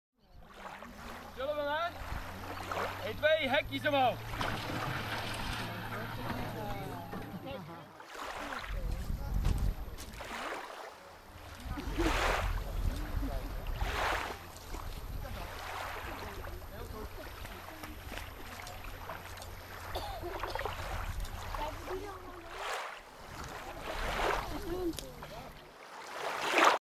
Zeehonden terug naar zee I Ecomare Texel
Wat een mooie dag! Vandaag konden vijf zeehonden uit onze opvang weer terug naar zee. De gewone zeehonden Klaartje, Pieter en Emmeke doken direct de golven in, maar de grijze zeehonden Hercules en Geert bleven nog even treuzelen.